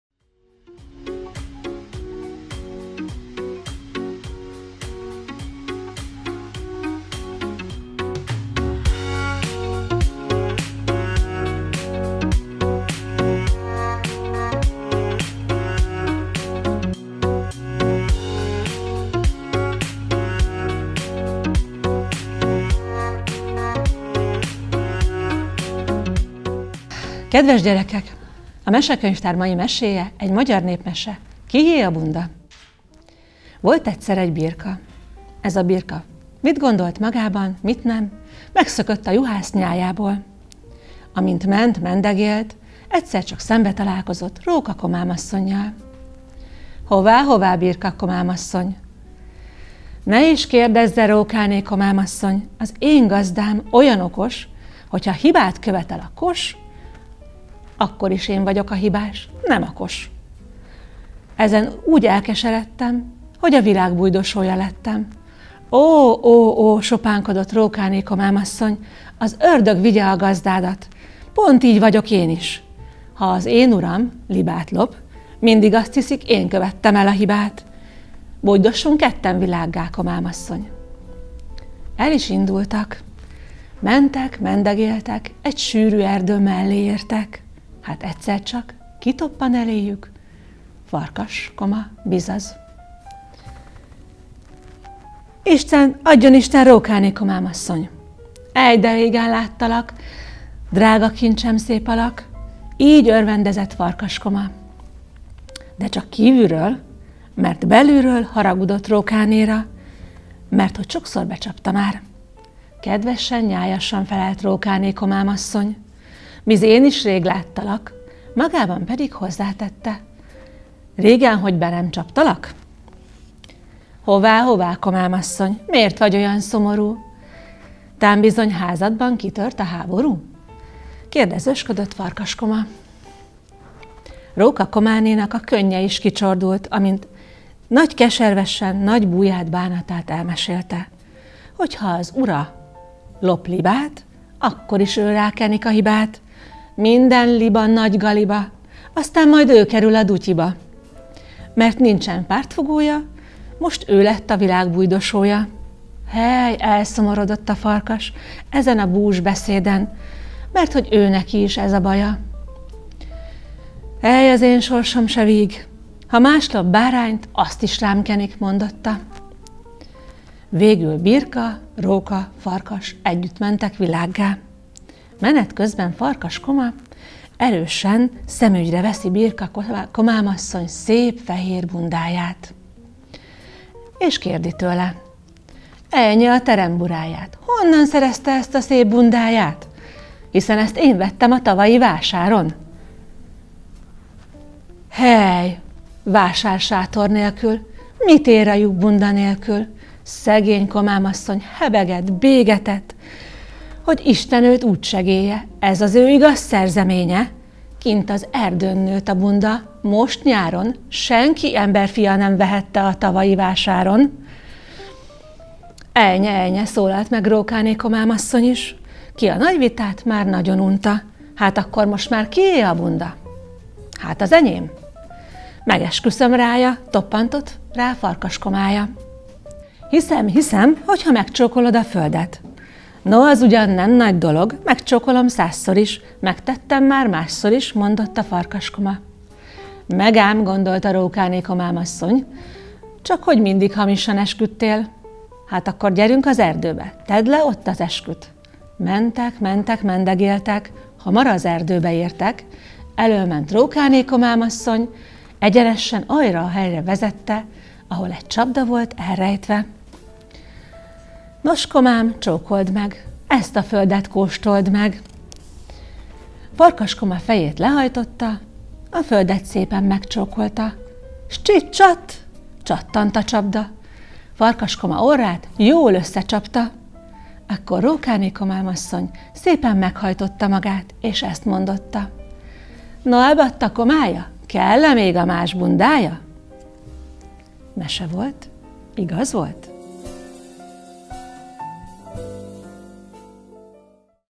Mesekönyvtár – Ritterné Rohoska Zsuzsanna mesél
A következő percekben a legkisebbekhez szeretnénk szólni. A Petőfi Sándor Városi Könyvtár munkatársai hétről-hétre meseolvasással kedveskednek gyerekeknek. A Kié a bunda? című mesét Ritterné Róska Zsuzsanna tolmácsolásában hallhatjátok.